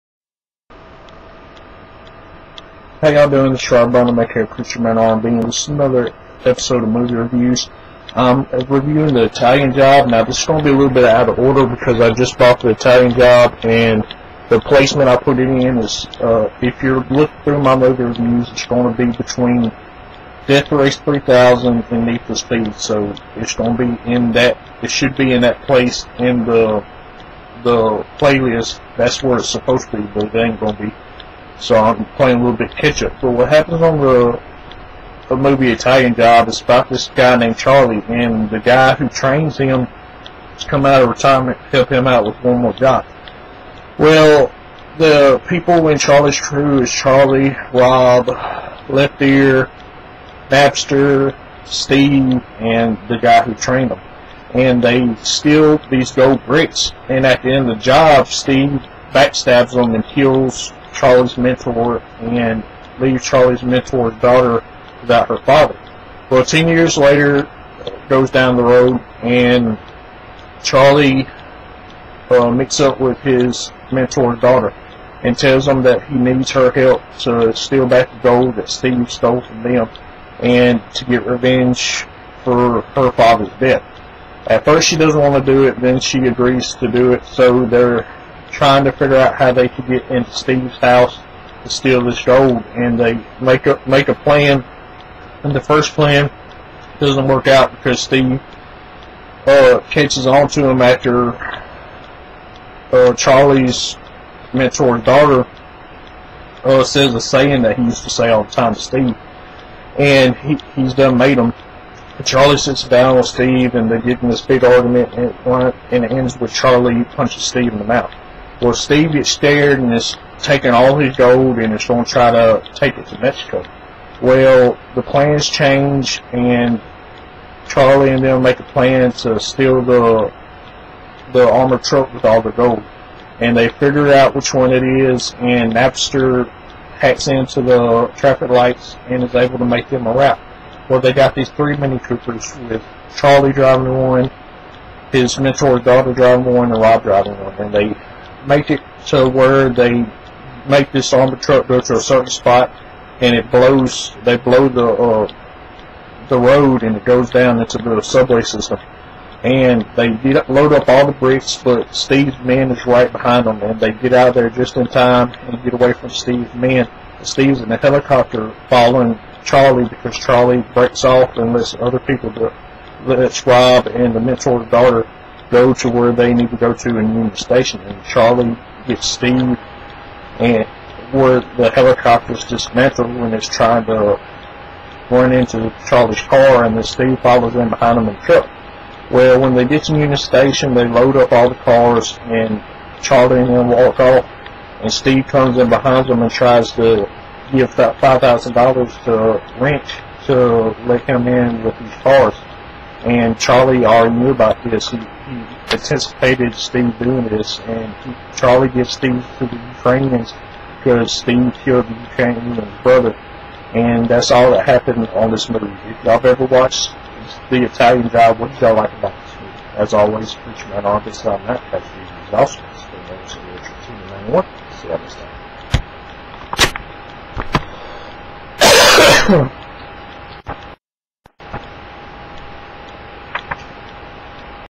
Movie reviews: The Italian Job
This Is another movie reviews and this time we are reviewing the Italian job.